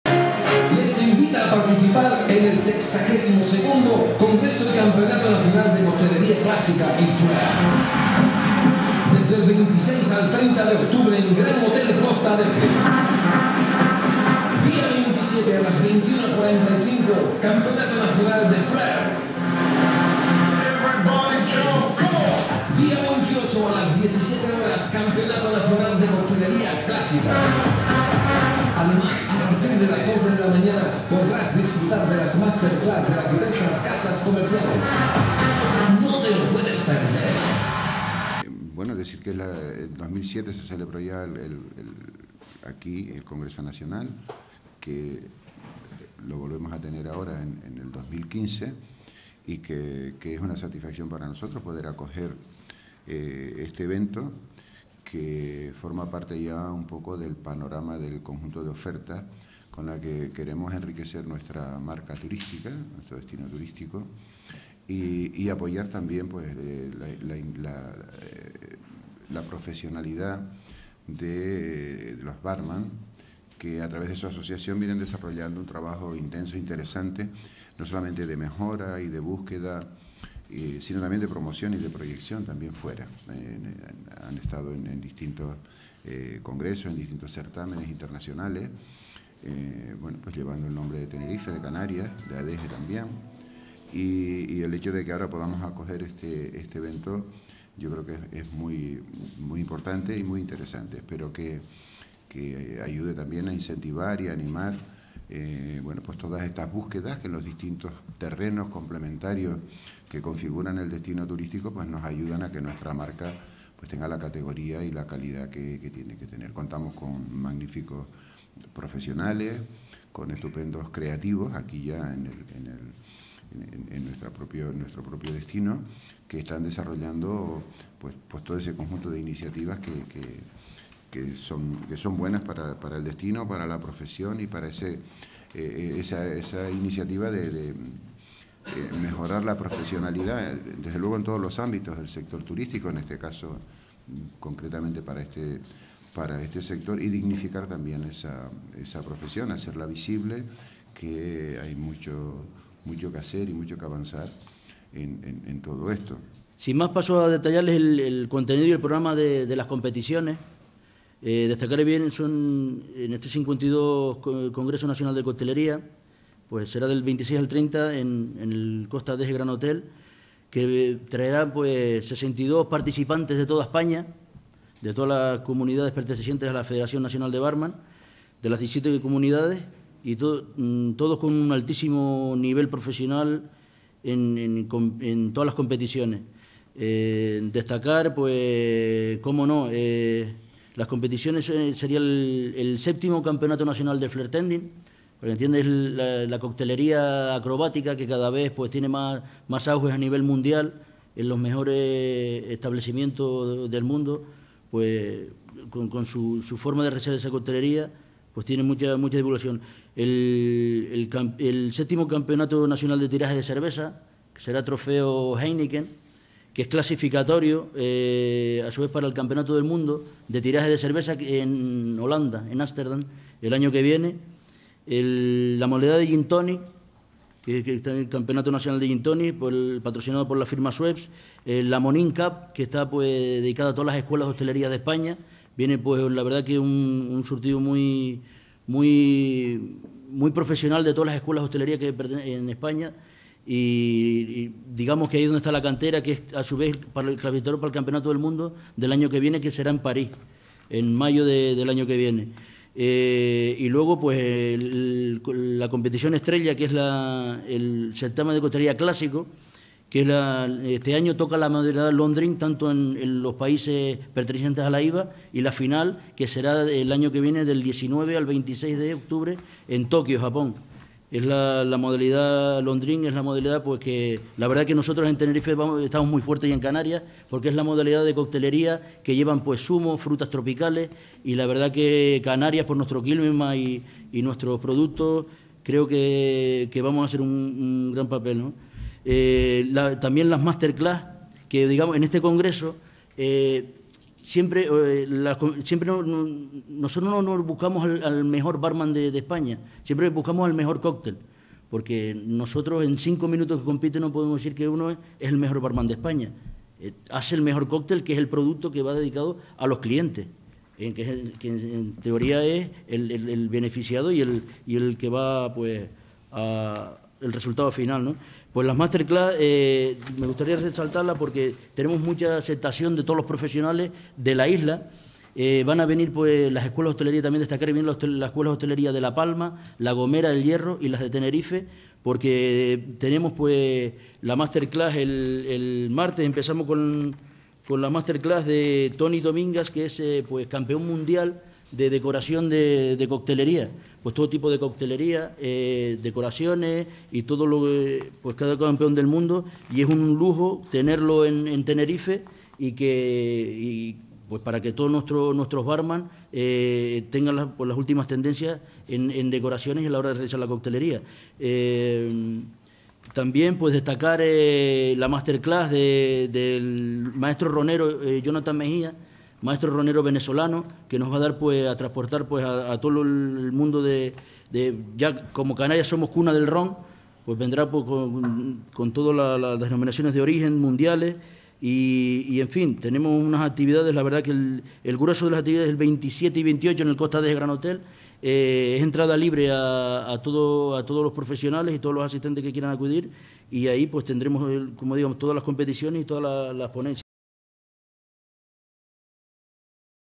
LXII Congreso Nacional de Coctelería Tenerife
Adeje a 22 de octubre de 2015
Los mejores barman de toda España se reunirán la próxima semana en Adeje

El Costa Adeje Gran Hotel será el escenario de esta importante convención y de la celebración de las pruebas nacionales de diferentes modalidades de coctelería.

El Municipio de Adeje acogerá la próxima semana el LXII Congreso Nacional de Coctelería
en el que se mostrarán las nuevas técnicas y tendencias en la elaboración de cócteles, de la mano de experimentados ‘bartenders’ procedentes de toda España, así como diferentes campeonatos nacional y pruebas valederas para participar en torneos europeos y mundiales.

El Centro de Desarrollo Turístico de Costa Adeje acogió ayer la presentación a los medios de esta importante cita organizada por la Asociación de Barman de Tenerife (ABT) en colaboración con el Ayuntamiento de Adeje, la Asociación Hotelera y Extrahotelera de Tenerife, La Palma  La Gomera y El Hierro (Ashotel), Turismo de Tenerife y la cadena GF Hoteles. Precisamente, el Costa Adeje Gran Hotel será el escenario de esta convención así como de todas las pruebas que se desarrollarán.

José Miguel Rodríguez Fraga, alcalde de Adeje, junto a Jorge Marichal, presidente de Ashotel, Ángel Tomás, director comercial de GF Hoteles y José Antonio Mesa, presidente de la ABT fueron los encargados de explicar en qué va a consistir la cita,  la importancia que tiene para el municipio, para el destino Costa Adeje y para el sector turístico.